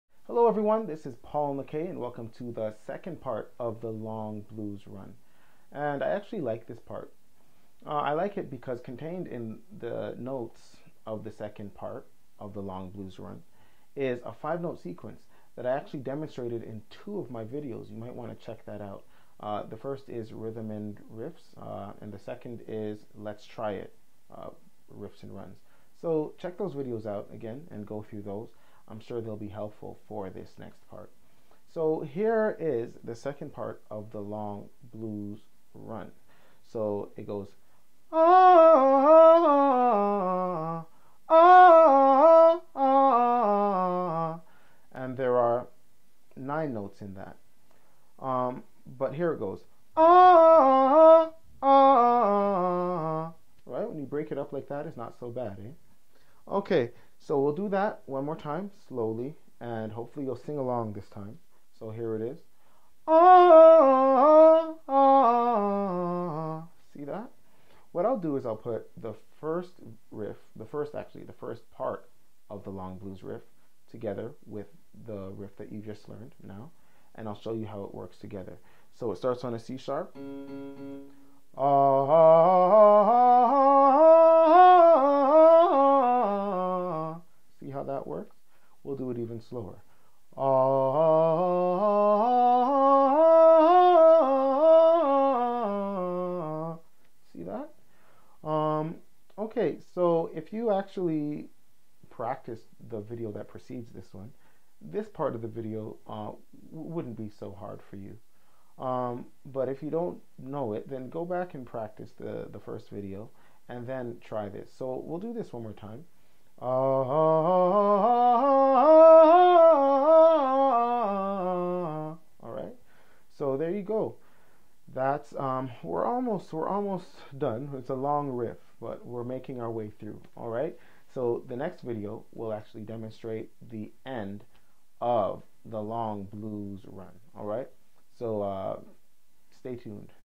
How to sing Riffs and Runs - BLUES SCALE Riff (part 2)
How to sing riffs.  How to sing runs.  Sing Riffs and runs the EASY way.
Riffs and runs, take you from being a ordinary singer extrordinary!  Learning riffs and runs, take a lot of practice but if you commit yourself to the work, and rehearse regularly, they become much easier.  Review this video to learn Riffs and runs easily.
Paul McKay proceeds to the Second part of the Blues scale riff